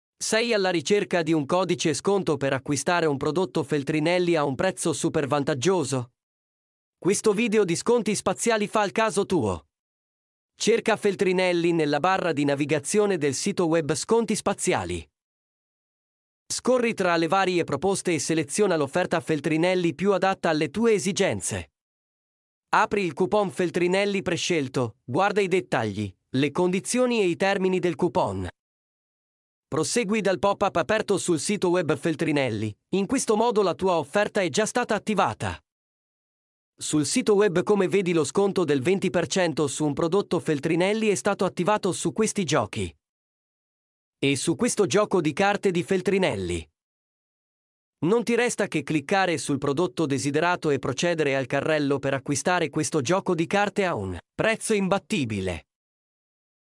Risparmia su libri e multimedia con il codice sconto Feltrinelli
Benvenuti nel canale dedicato a consigli su risparmio e offerte! In questo video, mostreremo come risparmiare su libri e multimedia utilizzando il codice sconto Feltrinelli su . La Feltrinelli è uno dei più grandi rivenditori di libri e prodotti multimediali in Italia, e grazie a questo codice sconto potrete ottenere incredibili vantaggi sugli acquisti.

Seguendo le indicazioni nel video, troverete facilmente link al codice sconto Feltrinelli che fa per voi e potrete utilizzare per ottenere uno sconto speciale su libri e prodotti multimediali. Approfittatene per arricchire la vostra collezione personale o fare dei regali speciali ai vostri cari.